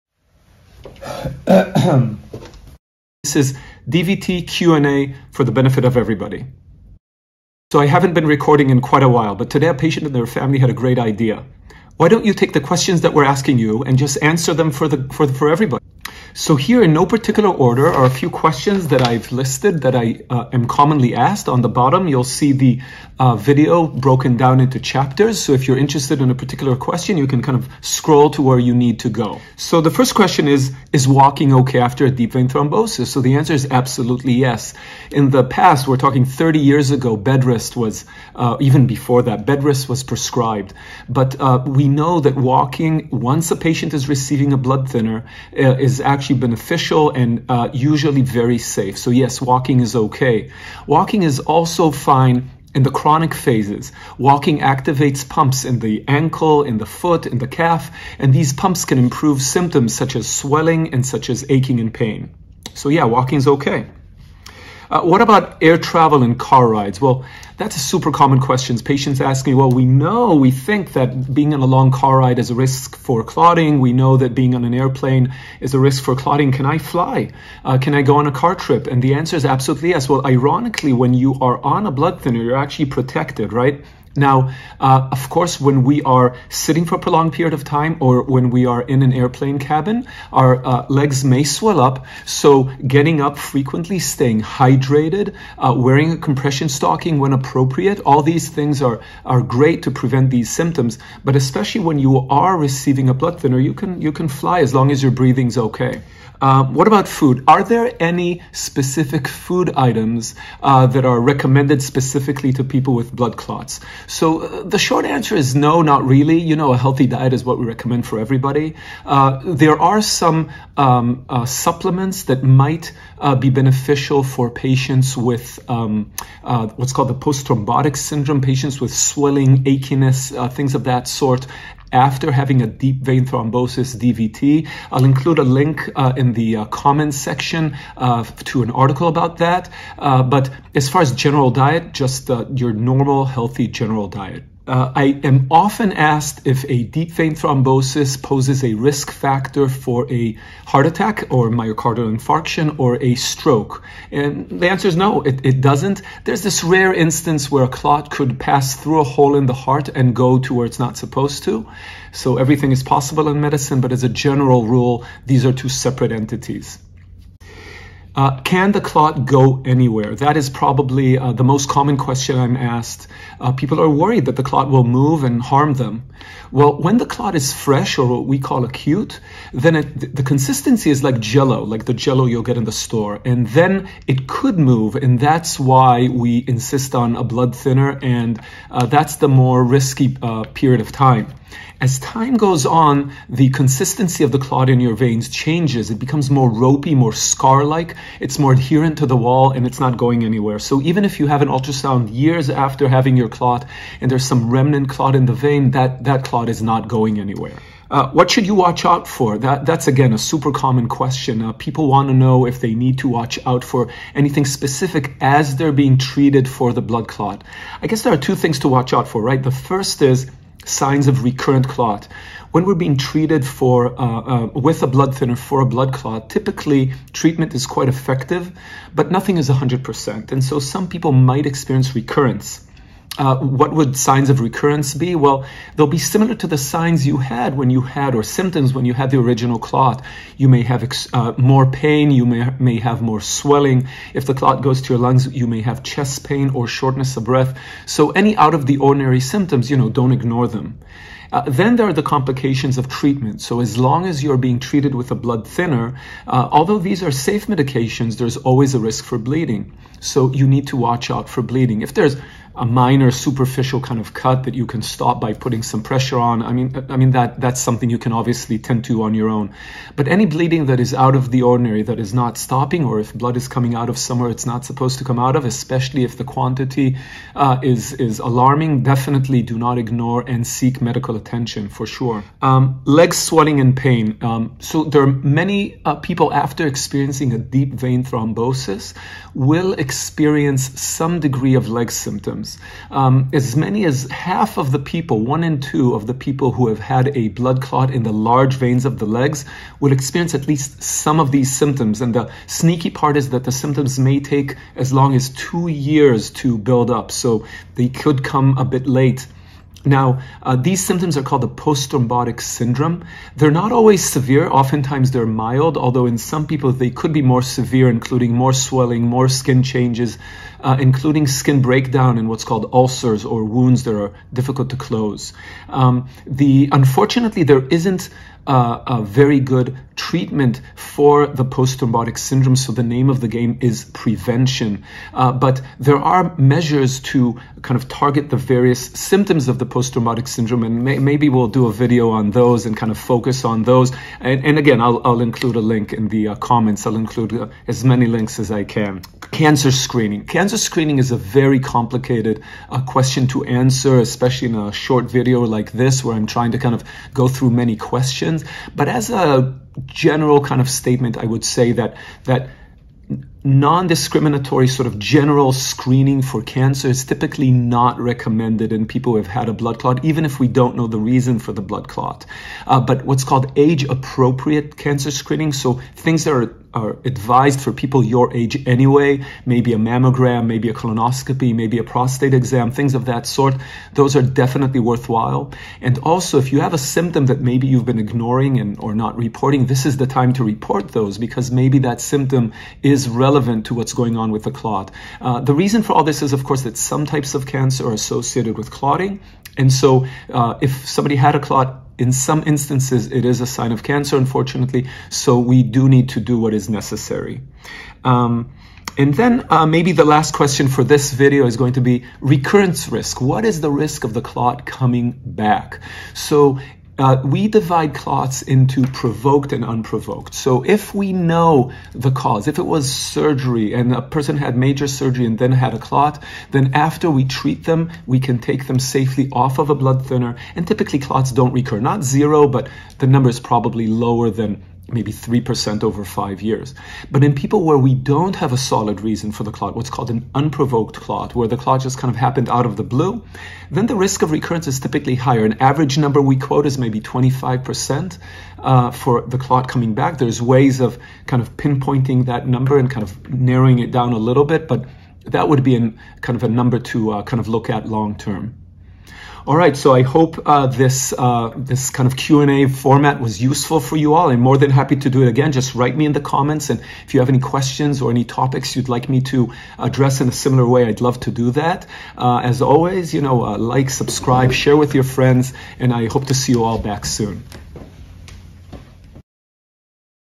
Deep Vein Thrombosis: Questions and Answers with Ido Weinberg, MD
Patients with deep vein thrombosis (DVT) have many practical questions. Can I walk? Can I travel? Are there any dietary restrictions? Will the clot move? Will the clot come back? Finding the answers to these questions can be hard. So I put together this video with a few common questions and answers about deep vein thrombosis (DVT). For medical advice, please consult your physician.

00:00 - Start
00:07 - Introduction
00:30 - Is walking okay after a DVT?
01:06 - Is travel safe after a DVT?
01:46 - Diet recommendations after DVT
02:24 - Heart attack and stroke risk
02:48 - Can clots move?
03:27 - What to watch out for
04:42 - Leg swelling and pain after DVT
05:49 - Cancer screening
06:47 - Will the clot come back?
07:39 - Summary

*Remember: This video is NOT meant as medical advice. There are many more factors that go into choosing a blood thinner. These are obviously prescription medications and you must always consult your physician.